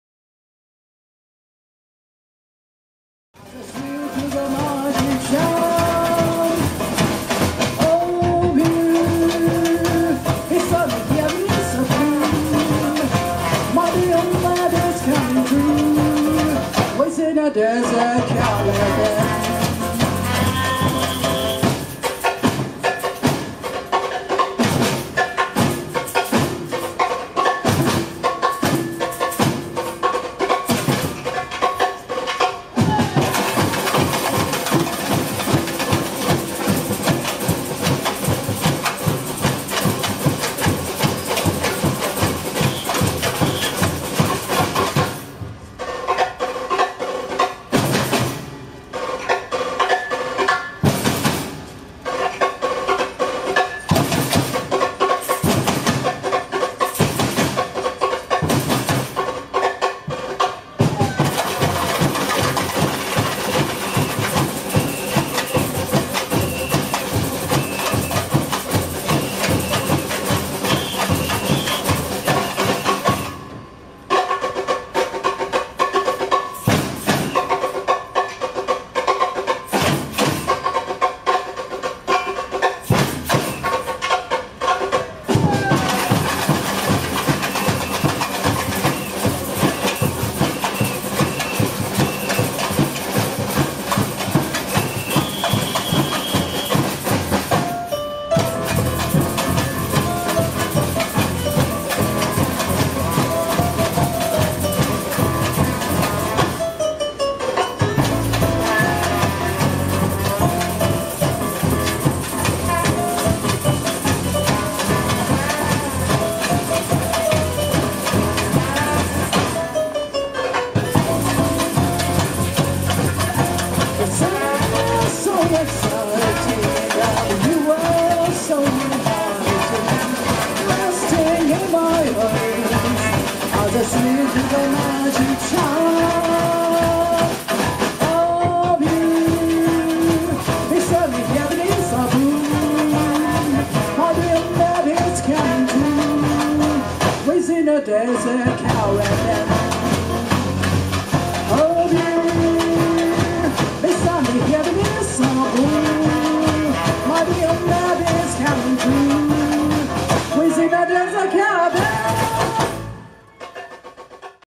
美しいサンバ(Samba) ２０１５神戸みなとまつり Part①
神戸メリケンパークで行われたＫｏｂｅ　Ｌｏｖｅ　Ｐｏｒｔみなとまつりでのサンバショーです（2015,7,19）